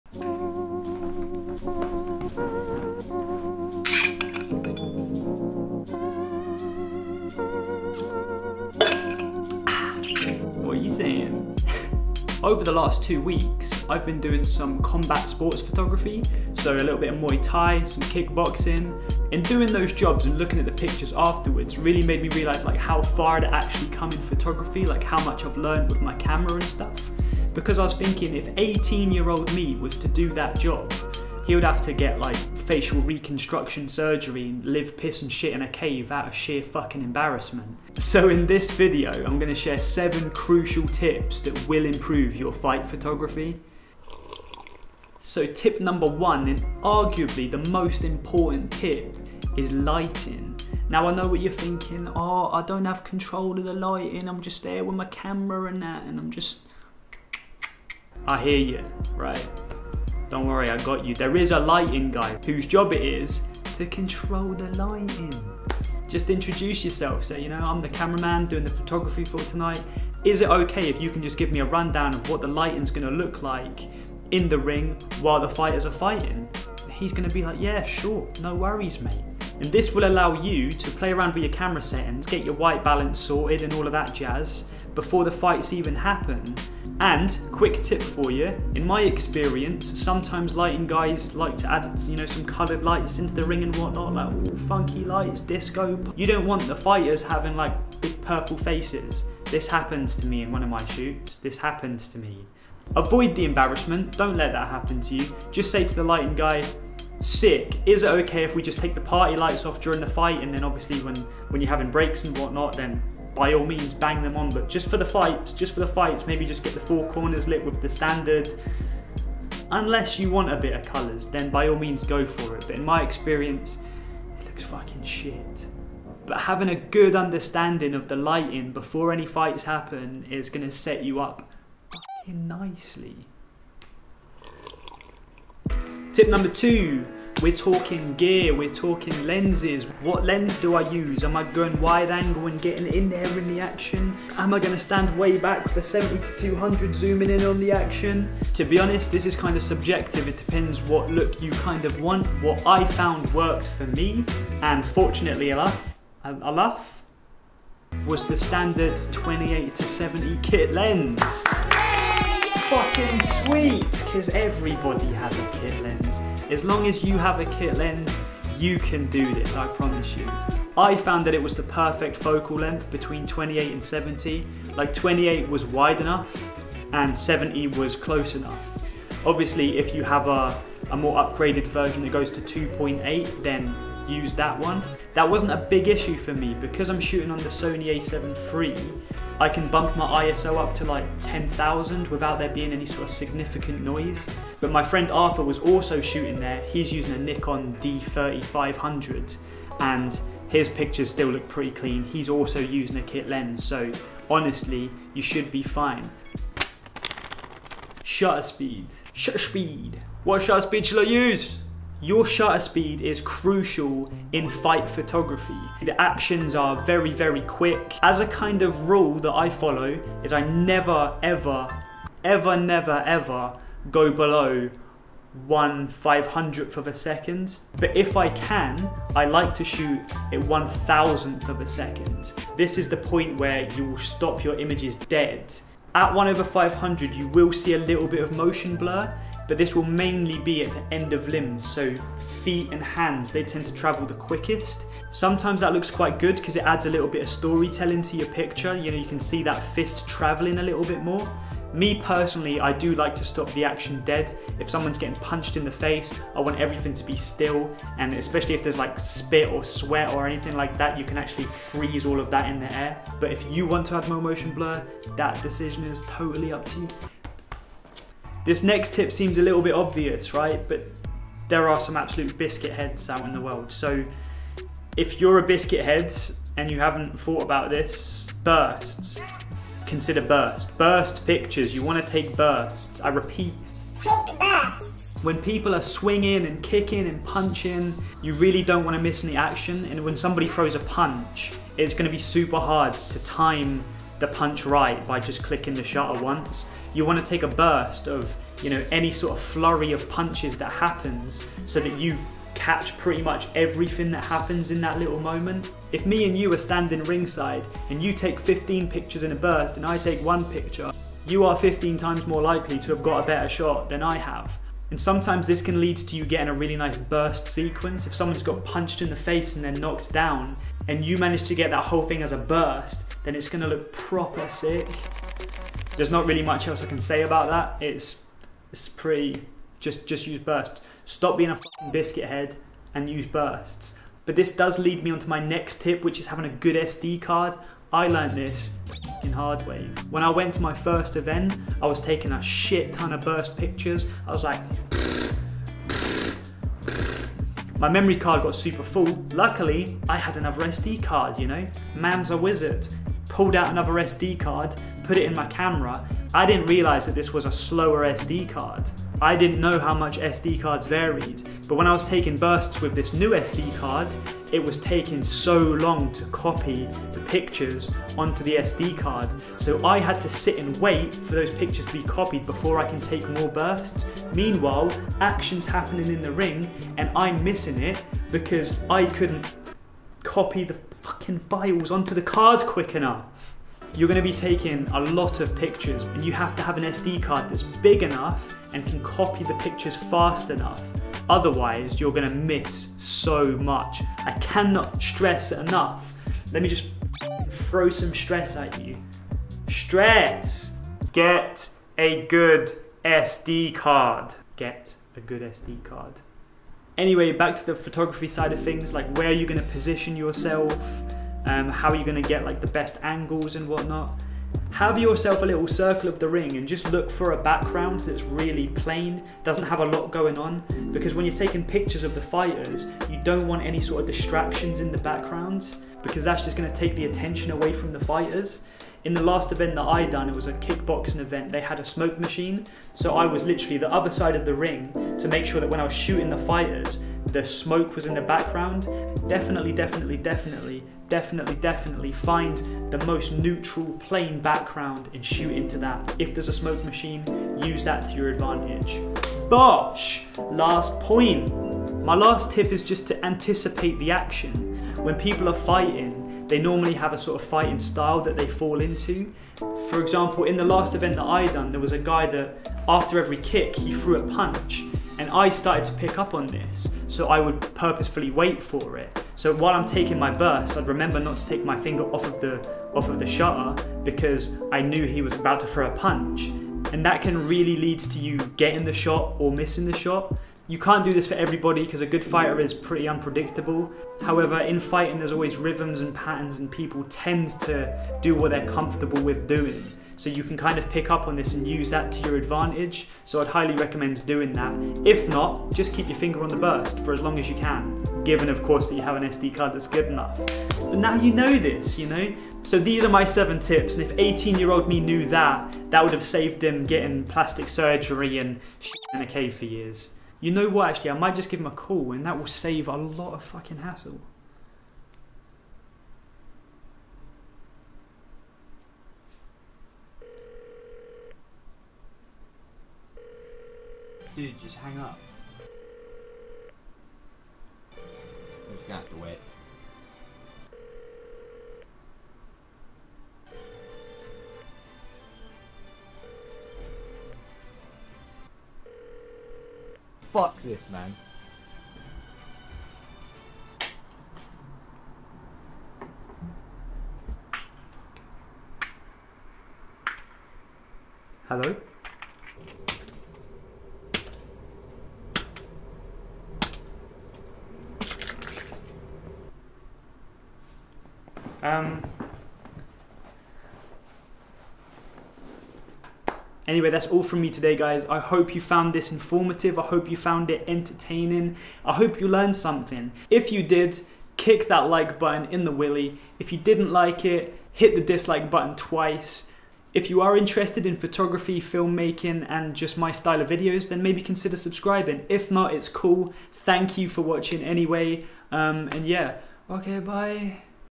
7 PHOTOGRAPHY TIPS that will IMPROVE your SPORTS PHOTOGRAPHY | Combat sports photography guide.
SPORTS PHOTOGRAPHY GUIDE and how to take better SPORTS PHOTOS | 7 Tips to improve SPORTS PHOTOGRAPHY. In this video I take you through some of the problems I faced when shooting sports events and some advice I wish I knew before starting sports photography.